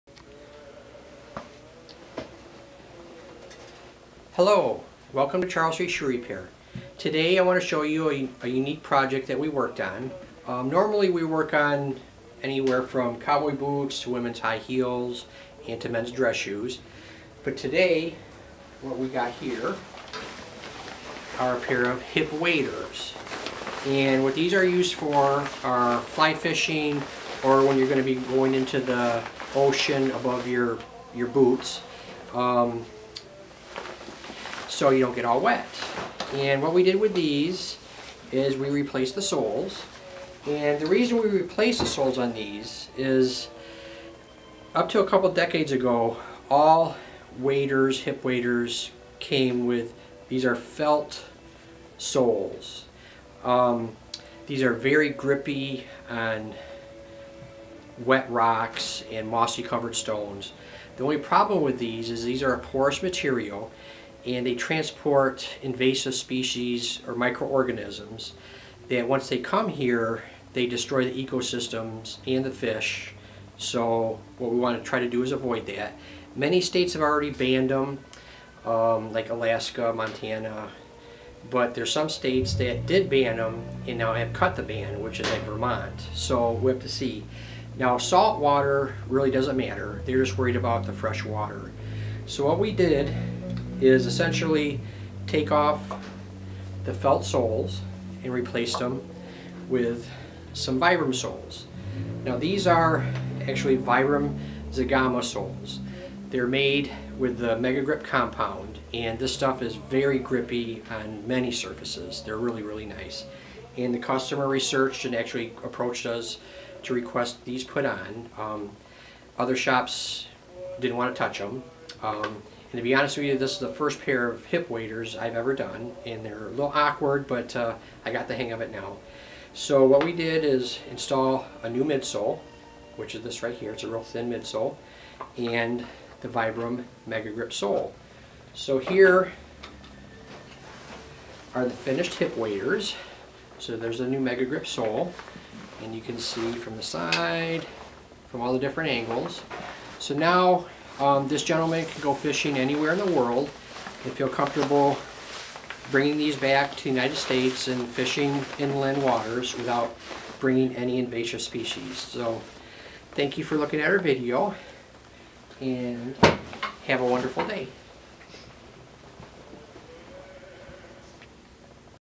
ORVIS HIP WADER RESOLE
Here we replaced the original felt soles with Vibram #1443 Zegama which are made with the Megagrip compound.